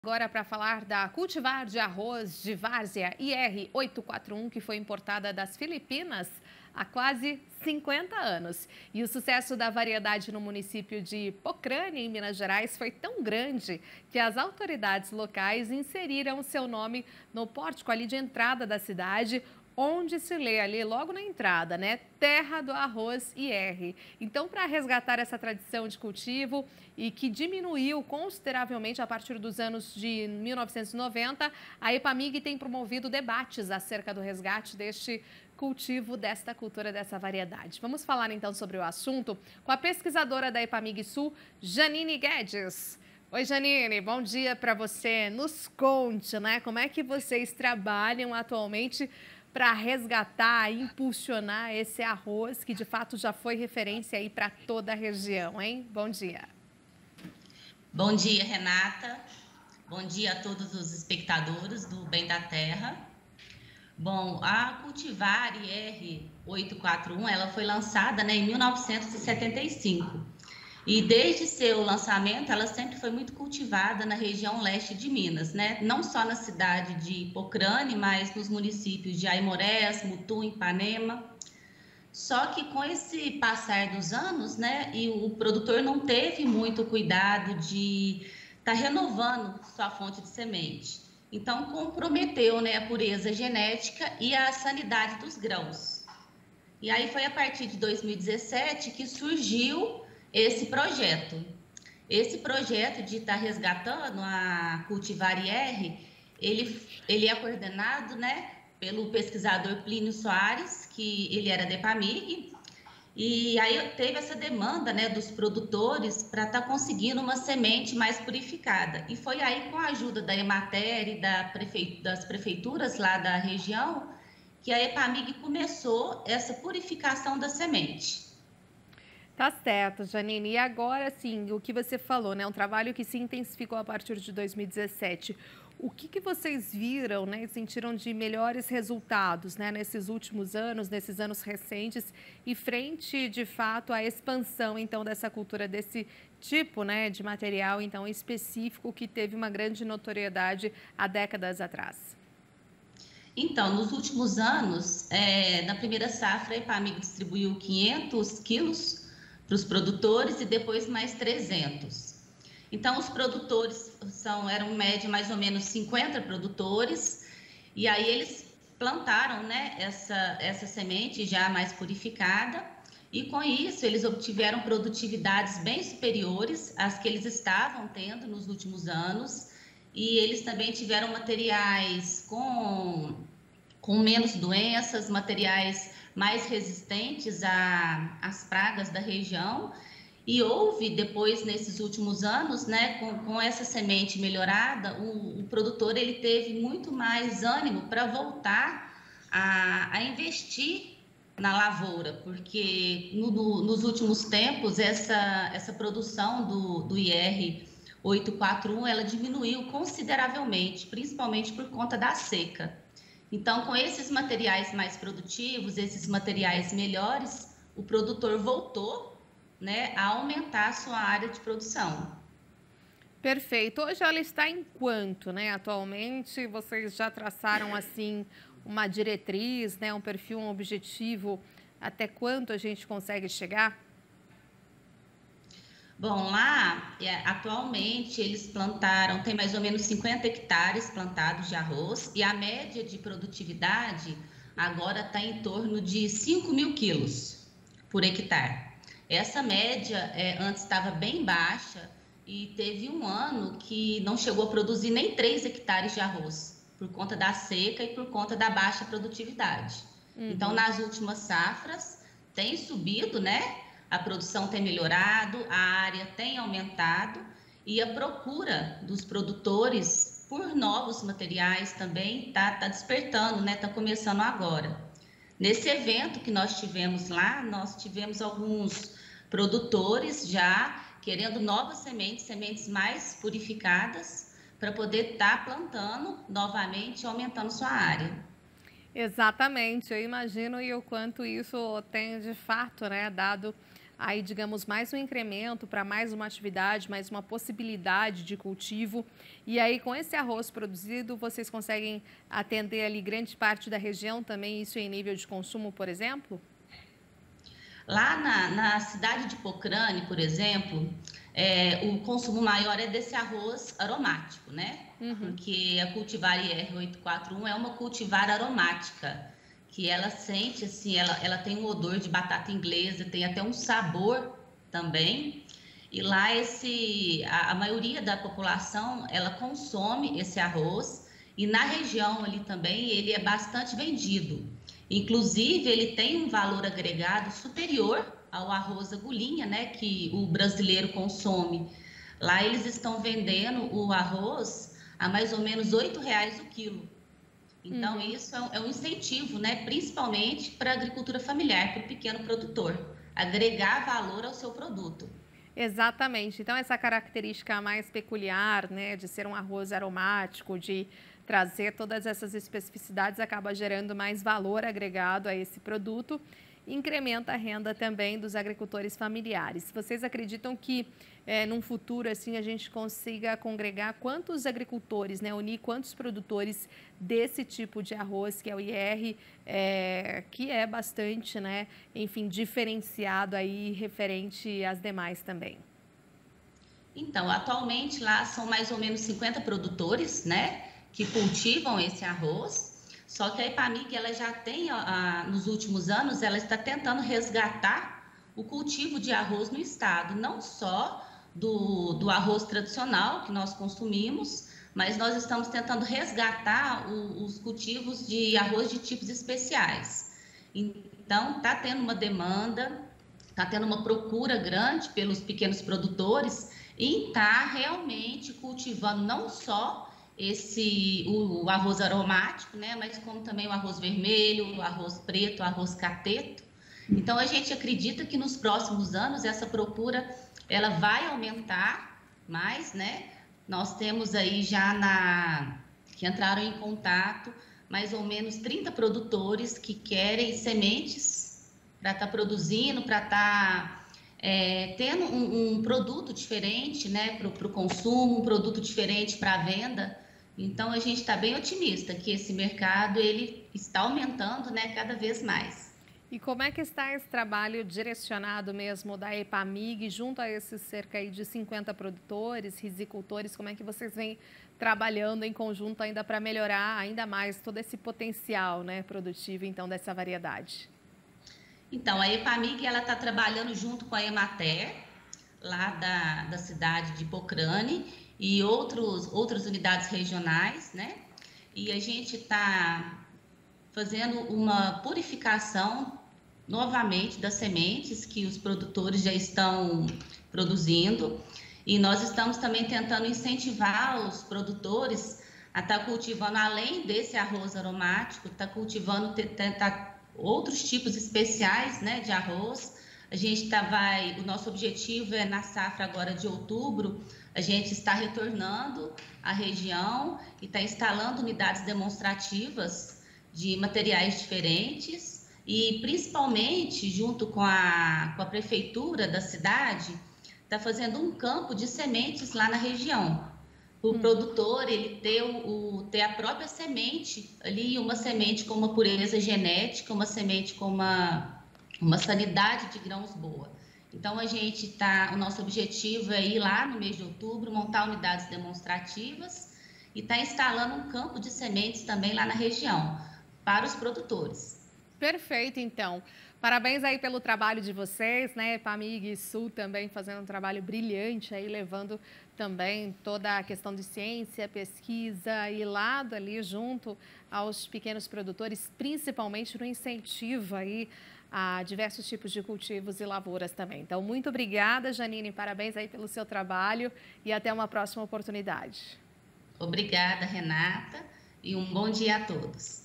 EPAMIG discute resgate do cultivo
A cultivar de arroz de várzea ir 841 foi importada das filipinas há quase 50 anos. O sucesso da variedade no município de Pocrane, em Minas Gerais, foi tão grande, que as autoridades locais inseriram seu nome no pórtico de entrada da cidade, onde lê-se: "terra do arroz ir". Para resgatar a tradição desse cultivo, que diminuiu consideravelmente a partir dos anos 1990, a EPAMIG tem promovido debates acerca do resgate deste cultivo. Para mais detalhes sobre esse assunto, entrevistamos a pesquisadora EPAMIG sul, Janine Guedes.

Confira!